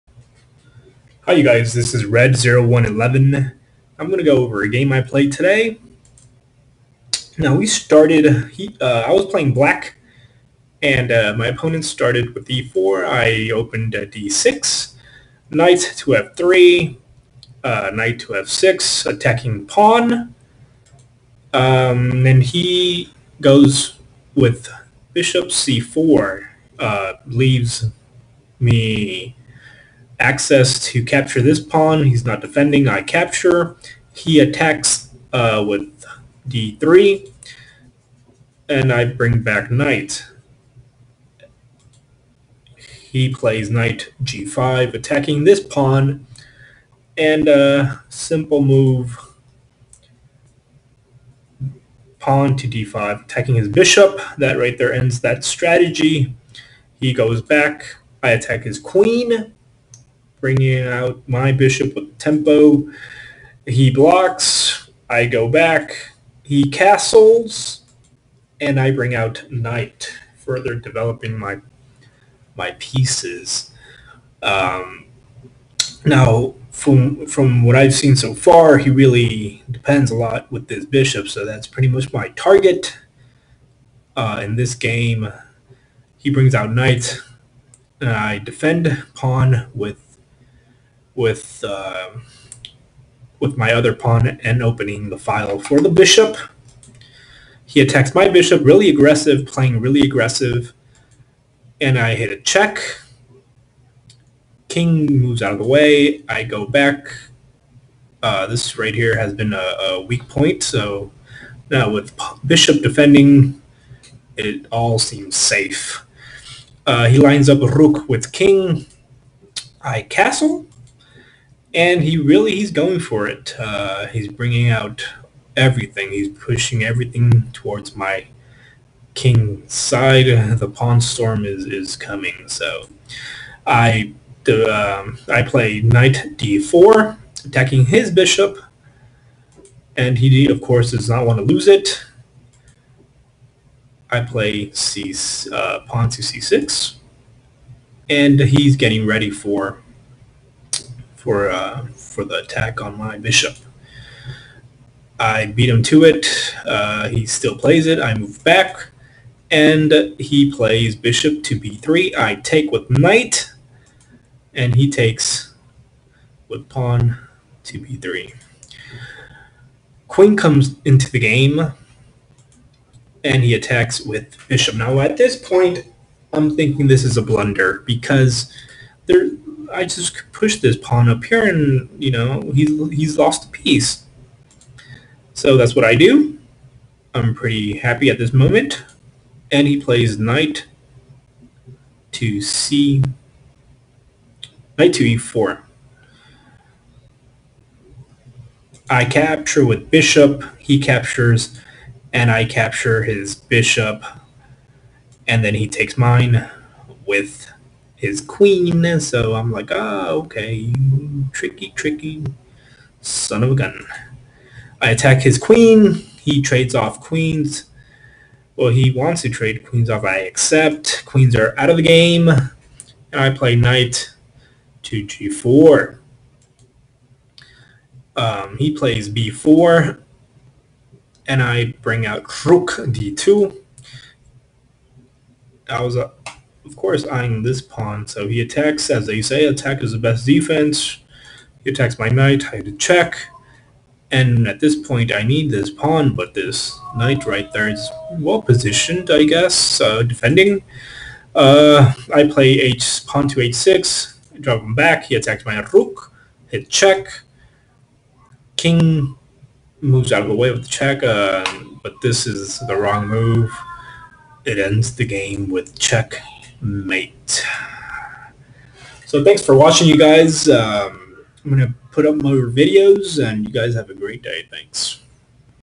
Checkmate that KING
This is a game I played online. Hope you enjoy the checkmate at the end.